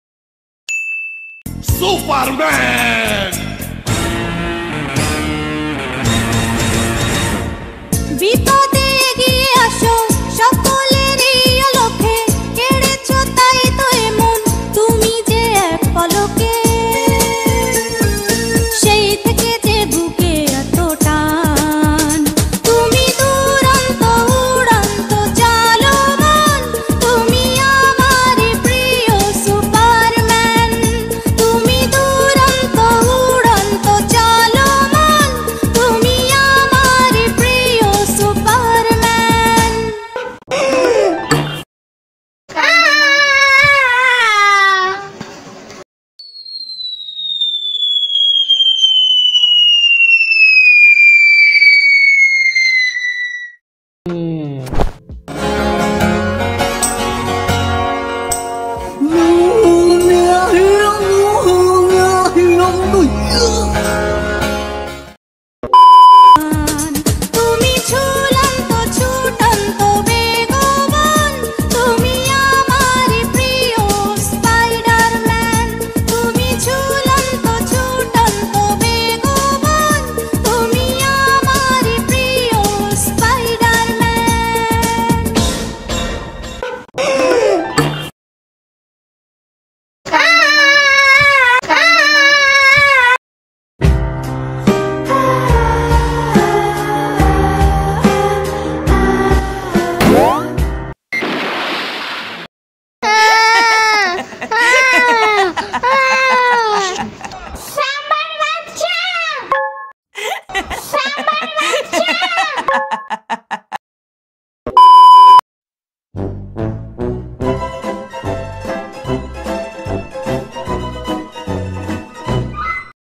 Superman Yamin II Fuad's cartoon II
Yamin যখন Superman হতে চায় ।Yamin & Samba funny video.
Hi, all I am a cartoonist and this one is a fanmade Samima Sraboni cartoon by Fuad.I made this video just for funny purpose.